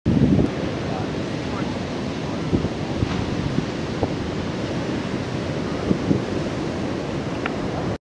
Lay Day Fun in Portugal with Leo Fioravanti
@leofioravanti taking care of Portuguese lay day business, as shot by Scotty Hammonds